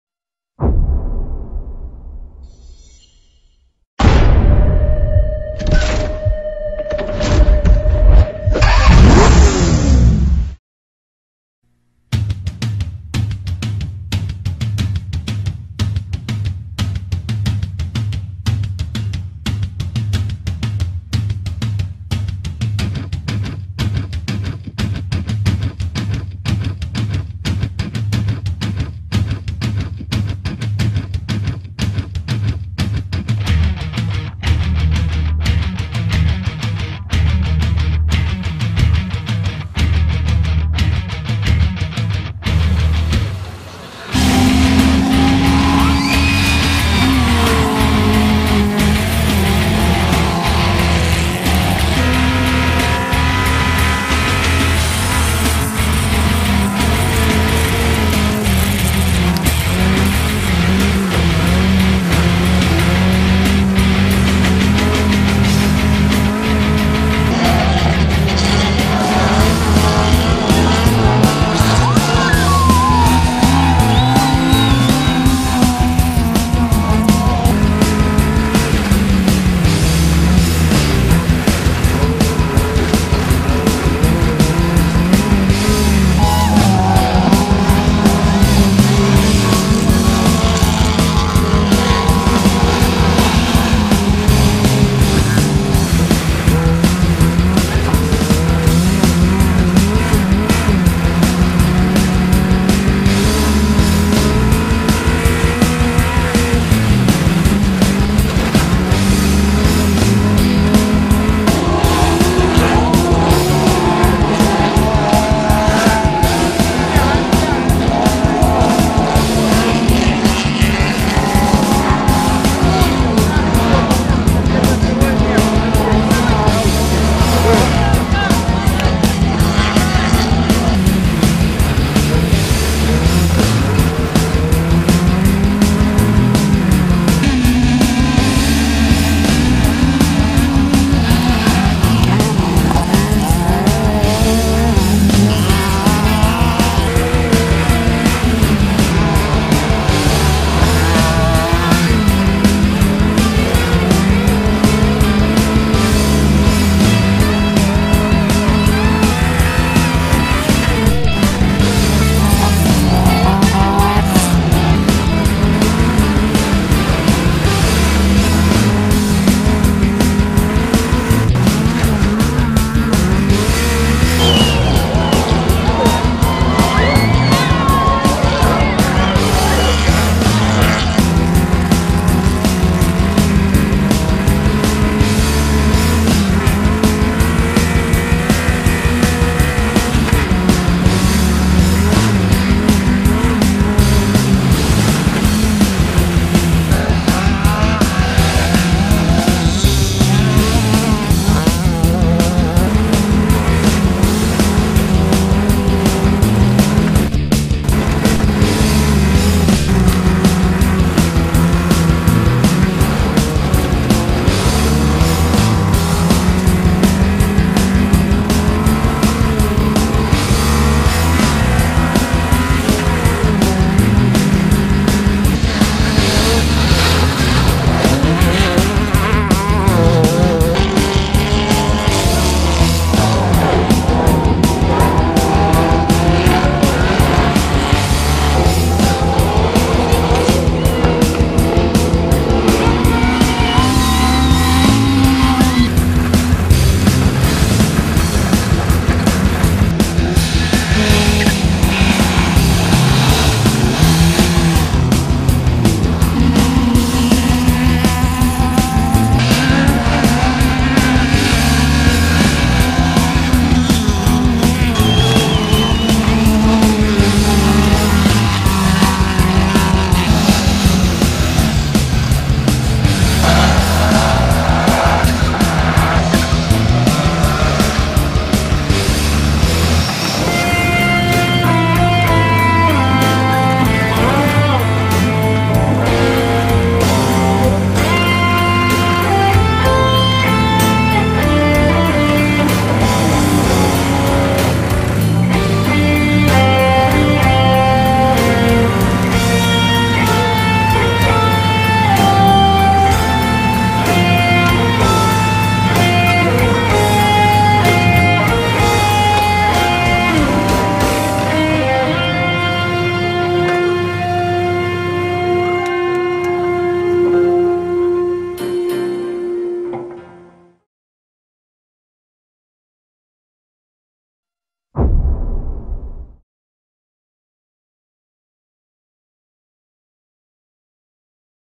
Auto Club Asist @ Autocross 2010 #3. Parteneri: BEMOL, Revista AUTOSALON. Moldova.
Auto Club Asist la Autocross Mai 2010, etapa 3. Federatia de Automobilism din Republica Moldova (FARM). Partener petrolier: BEMOL. Partener media: Revista AUTOSALON.